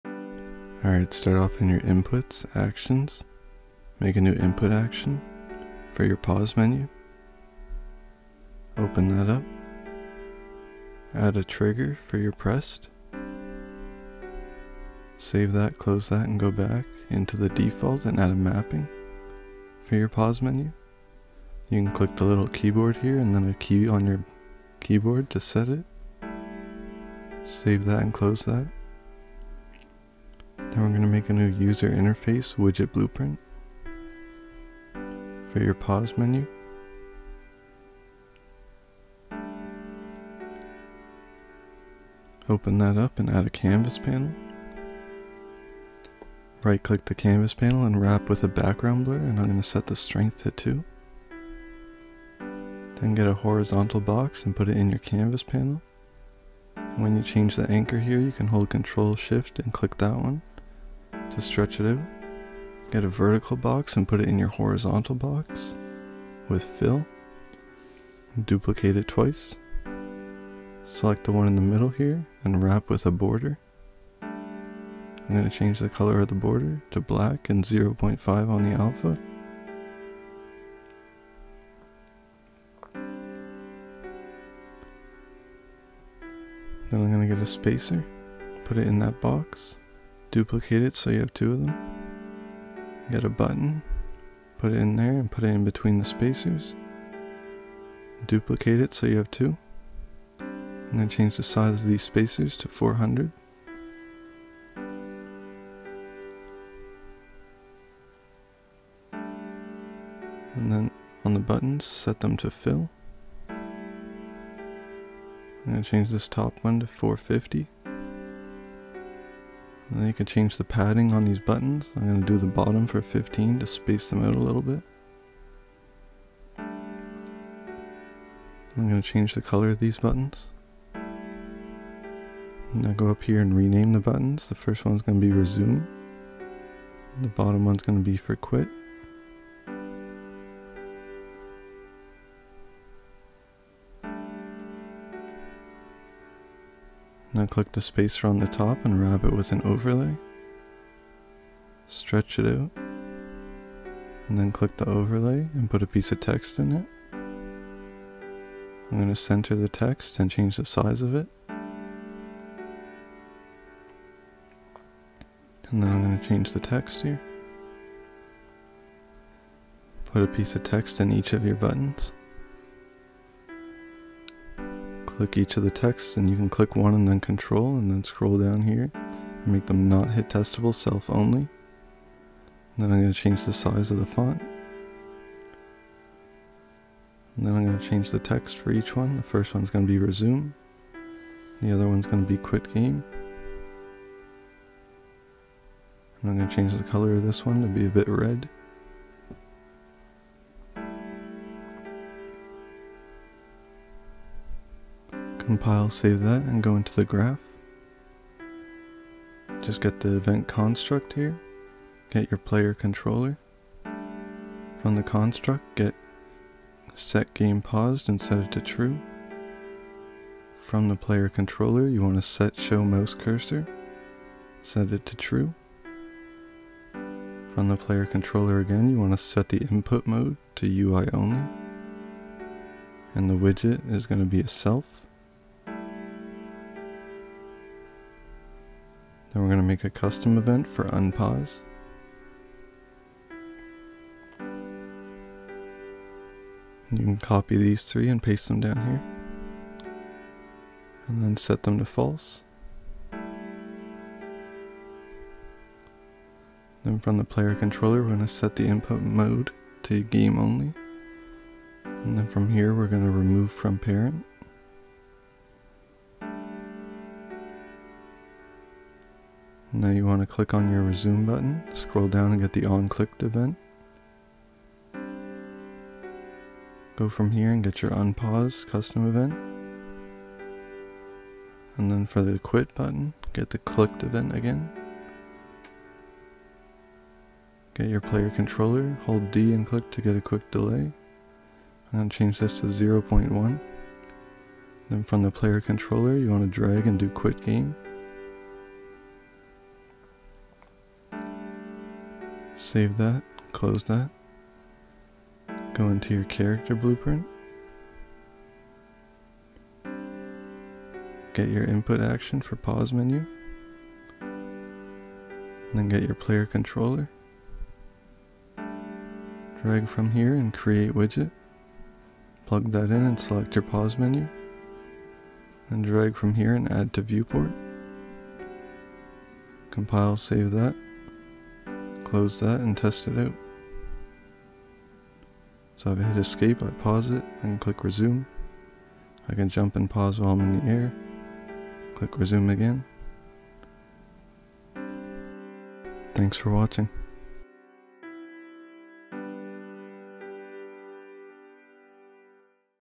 How To Make A Pause Menu | Unreal Engine 5 Tutorial
Skip to 3:26 if you already designed a pause menu. In this tutorial, we will be learning how to setup a pause menu for your Unreal Engine 5 project using simple techniques. In just a few easy steps, you will have a pause menu. We will be using Blueprints to Follow along with step-by-step instructions and you will have a working pause menu in no time! UE5 Tutorial, Unreal Engine Tutorial.